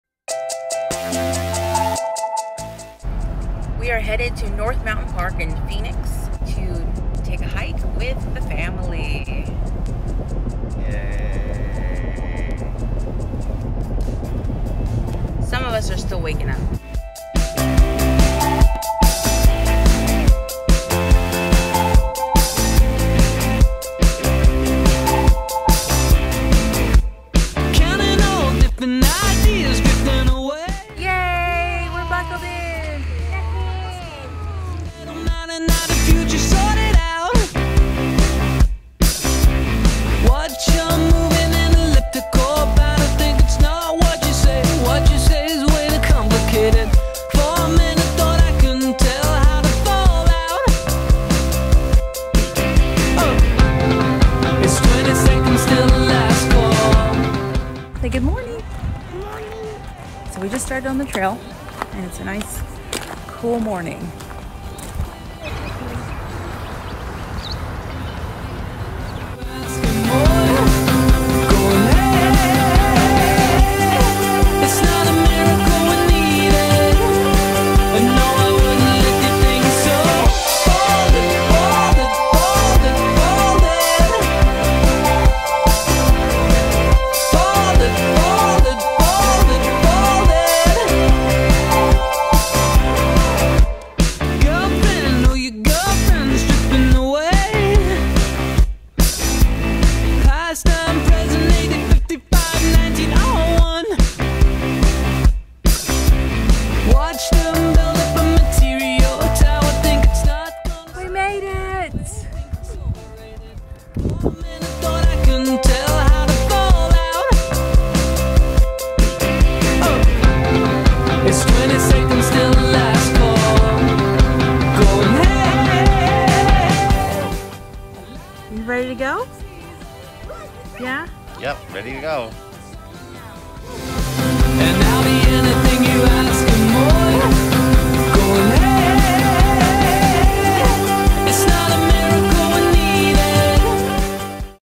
Family Hike Up North Mountain Park In Phoenix
We rose with the sun and hiked up what I like to call Central Mountain in Phoenix, Arizona. The views never disappoint.